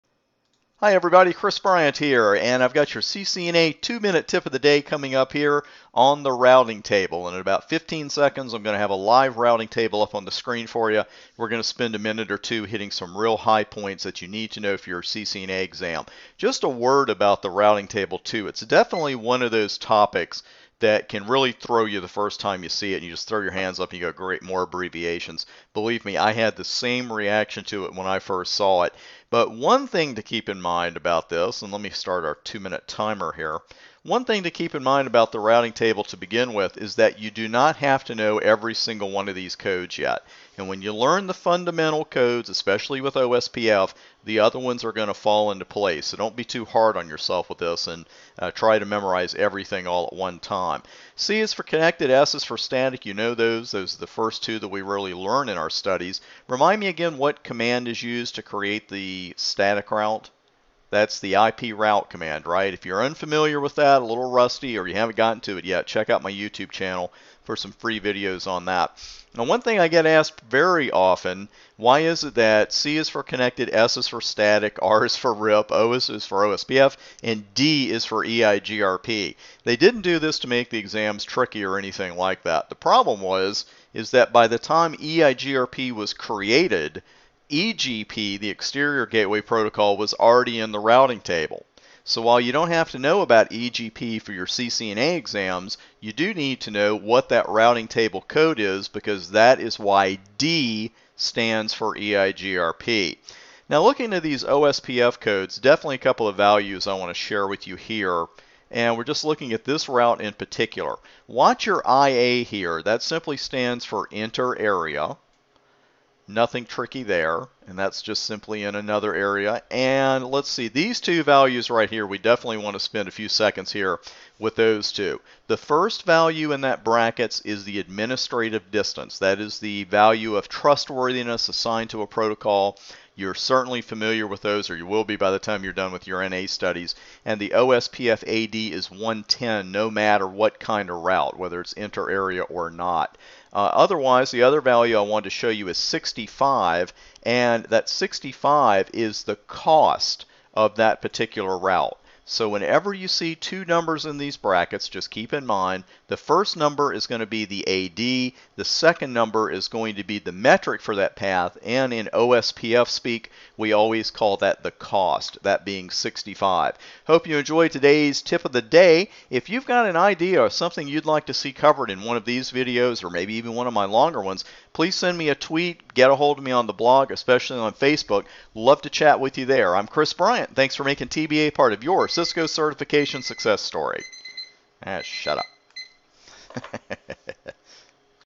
CCNA Video Tip Of The Day: OSPF And EIGRP Routes
for over 450 additional free videos and tutorials to help you pass your certification exams!

In today's CCNA Video Tip Of The Day, we'll take a close look at some routing table codes and entries, and I'll share a success tip with you as well.   Enjoy, and thanks for making TBA part of your CCNA success story!

Chris Bryant
CCIE #12933
"The Computer Certification Bulldog"